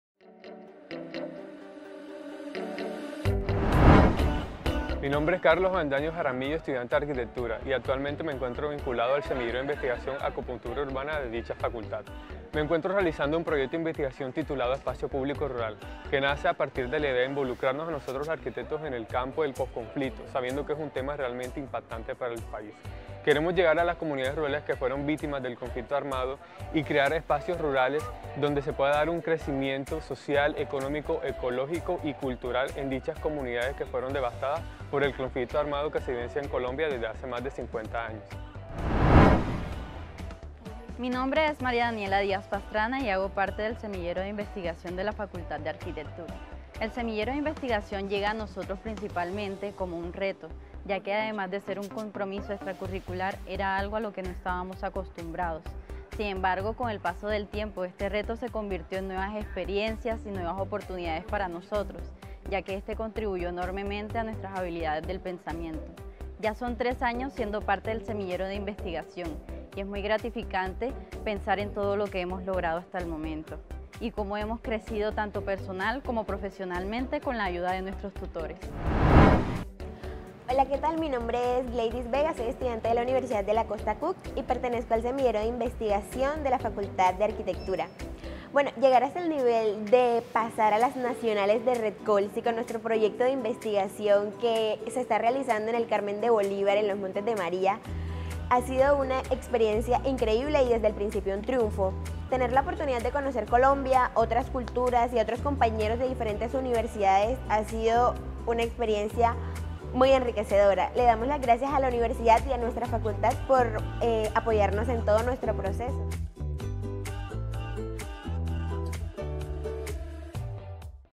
María Mágica. Una propuesta de Espacio Público Rural para el municipio del Carmen de Bolívar.
Los estudiantes Andrea Ramírez, Miguel Ortega y Jean Simmonds, del Departamento de Arquitectura y Diseño desarrollan una investigación sobre la problemática del SINHOGARISMO, con el diseño de un prototipo arquitectónico sostenible denominado «HABITÁCULO», un elemento que integra Hábitat y Producción Económica, en respuesta a dos de los derechos fundamentales de todos ser humano: Vivienda y Economía. Un trabajo que se fundamenta en la inmersión social de los estudiantes y se eleva al ejercicio propio del diseño. Un trabajo Dirigido por el Arquitecto Samuel Esteban Padilla Llano, Decano del Departamento de Arquitectura y Diseño en colaboración con, Mauricio Cabas G, Emerson Martínez Palacios y la Arquitecta PhD. Andrea Fernandez Covarruvias (Chile) como asesora Internaciónal. A arquitectura